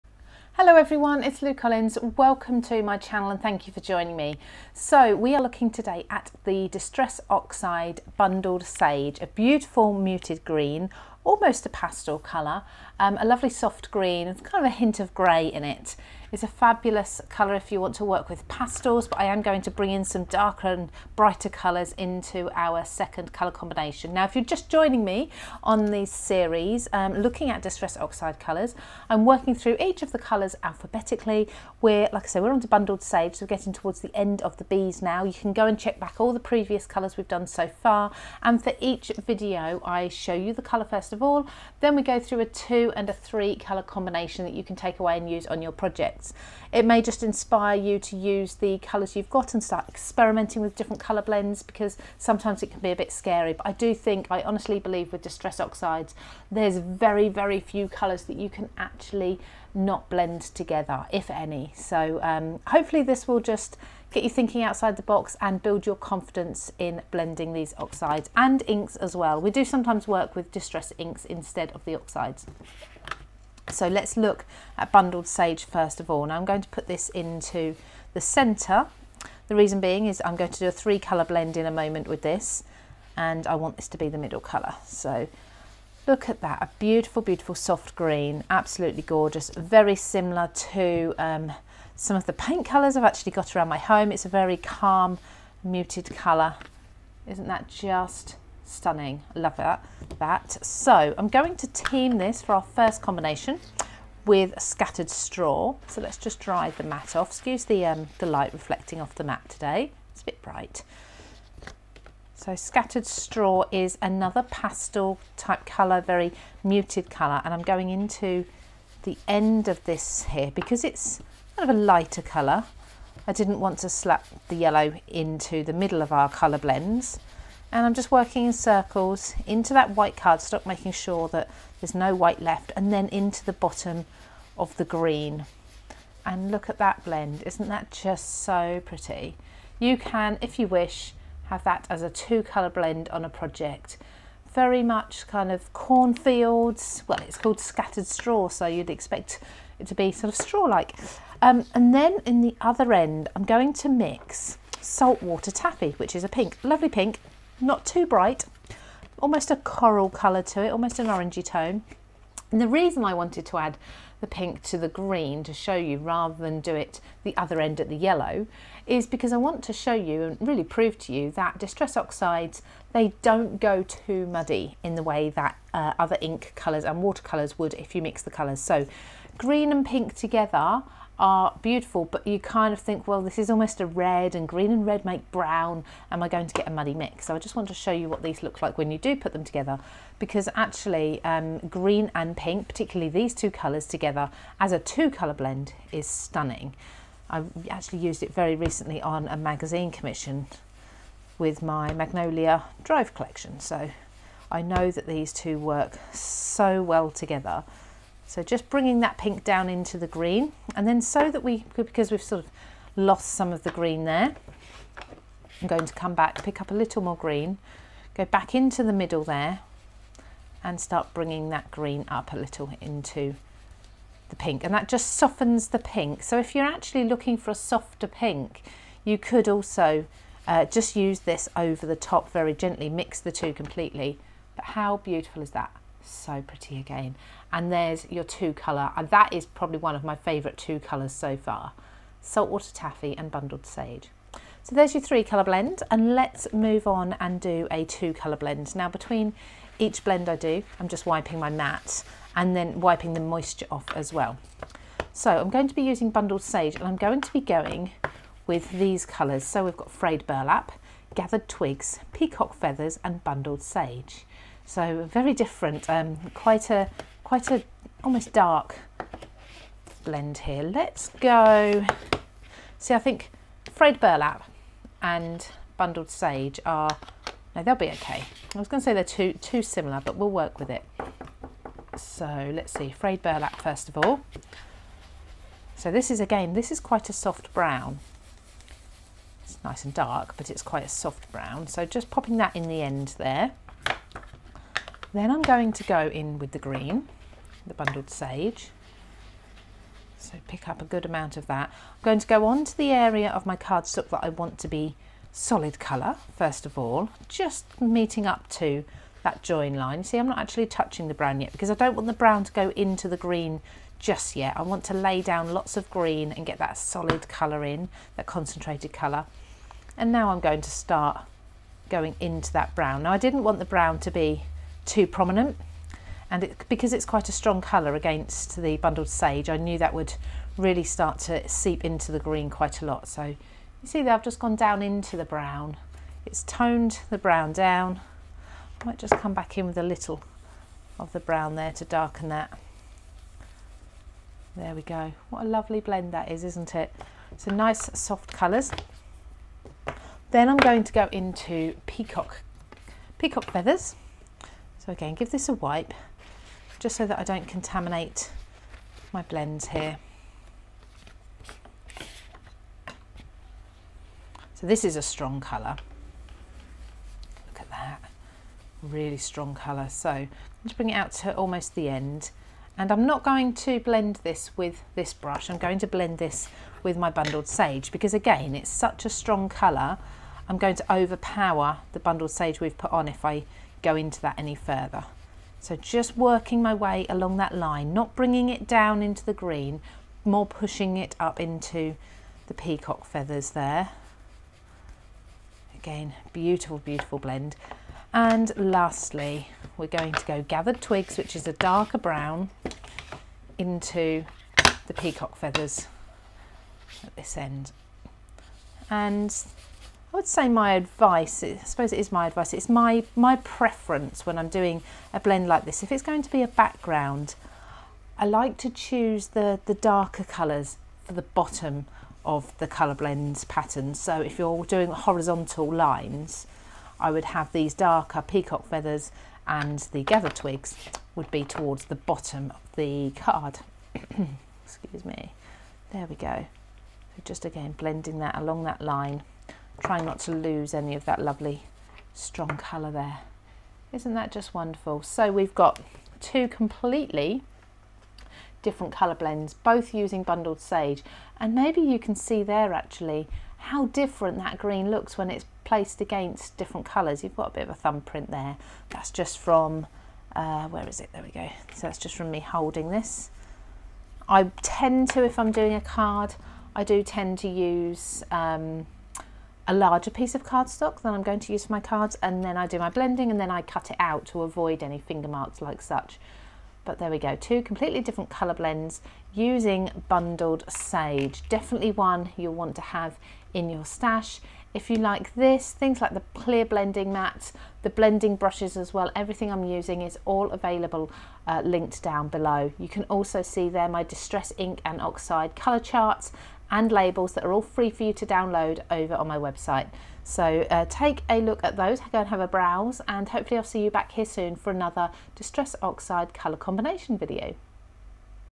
DISTRESS OXIDE COLOUR COMBINATIONS - Bundled Sage
Part of a new series exploring each of the Ranger Tim Holtz Distress Ink & Oxide colours and discovering colour combinations with each.

This video is looking at Bundled Sage, a beautiful soft  and pale green.
All videos in this series will be uploaded alphabetically, according to the colour name. Please see the Distress Oxide Colour Combinations playlist for more.

You will find the Distress Ink & Oxide colour chart and labels FREE for you to download on my website here